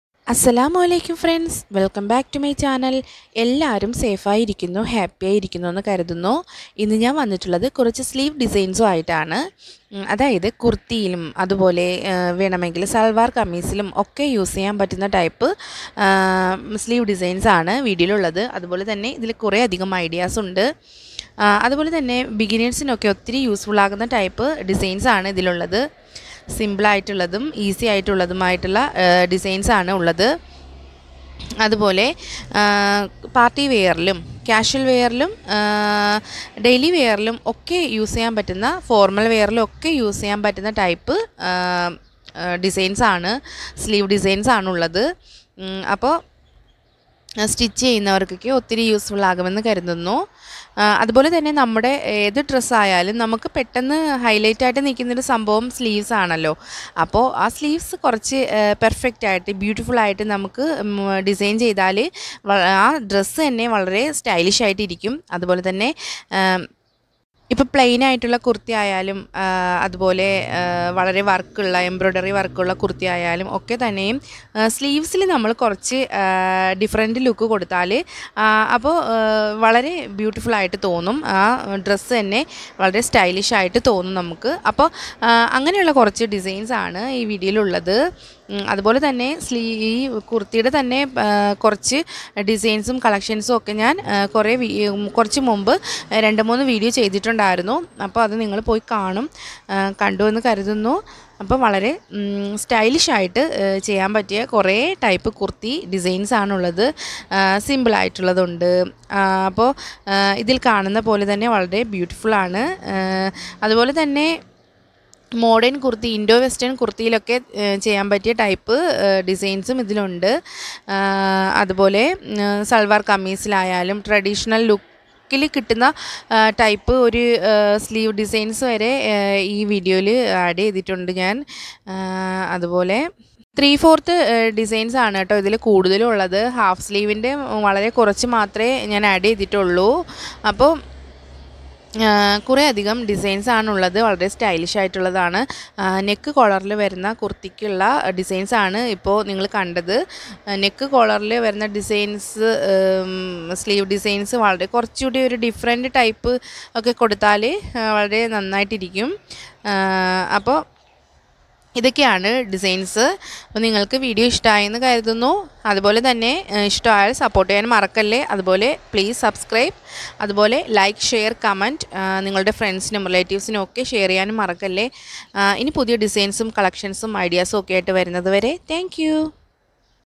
Latest Kurti Sleeve Designs & Ideas/latest beautiful kurti/salwar kameez sleeves/top designs 2021
♥️Assalamo alaikkom dear viewers and subscribers!today I'm going to show you some Latest kurti sleeve designs& ideas for beginners 🥰

🎀 Content used for informational & reference purposes only, Nothing to do with promotion🎀

🦋PLEASE NOTE:🦋
Some products given in the link might not be same but will be similar products.